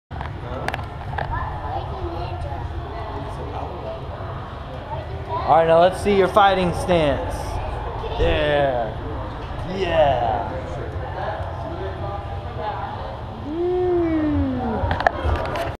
[Behind the Scenes 2]- Power Ranger visits the kids @ Ultimate Training Systems - Metroflex SA
Thanks to Ryan Turney aka Power Ranger